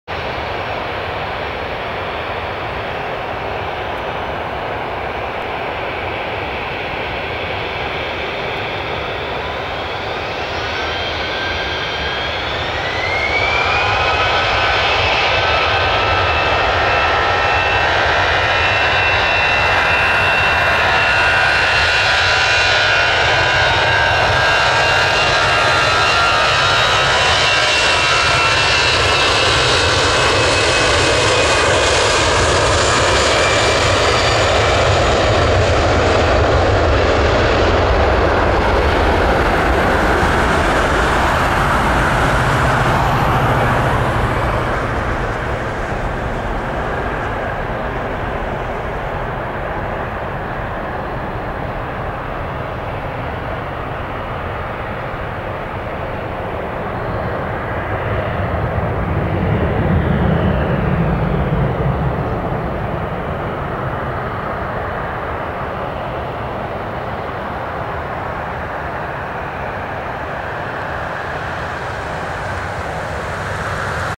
Frontier Airlines N221FR A320 Takeoff Portland Airport (PDX)
Flight: FFT796 PDX-DEN